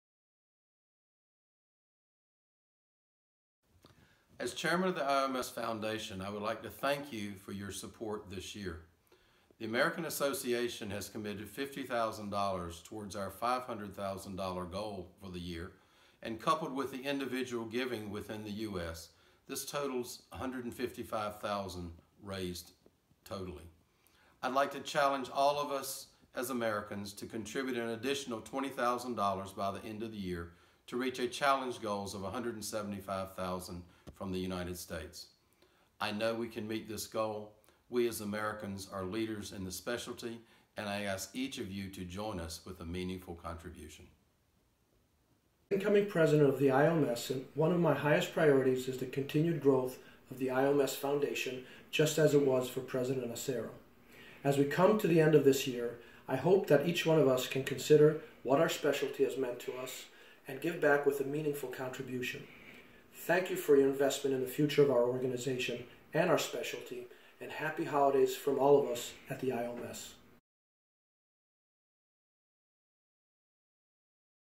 The IAOMS Foundation Appeal Challenge for U.S. Members
Join w/other IAOMS U.S. members and help the IAOMS Foundation meet its $500,000 Annual Appeal goal for 2017.  Donate today and help educate the next generation of OMF surgeons.